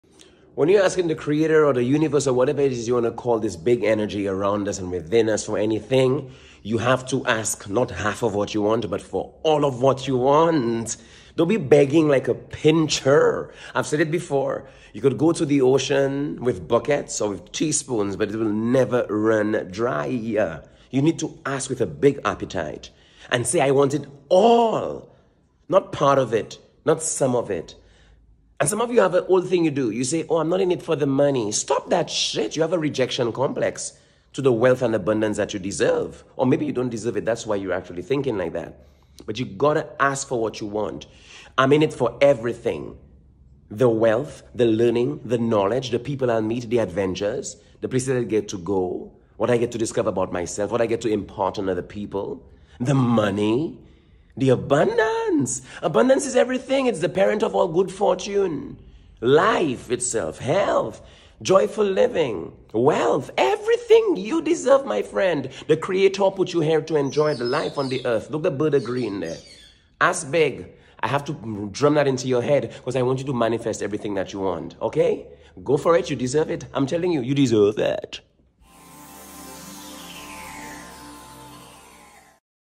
Ask for what YOU want!
When you set your intentions and desires, do it on a grand scale. The Creator, the force that governs the universe, is boundless and infinitely generous. There is no need to hold back or limit yourself. Envision your dreams without boundaries, and pursue every one of them with unwavering determination. The universe is vast and abundant, ready to support you in manifesting your deepest aspirations. Believe in your potential and trust that the universe has your back, guiding you toward a life of limitless possibilities. ✨💫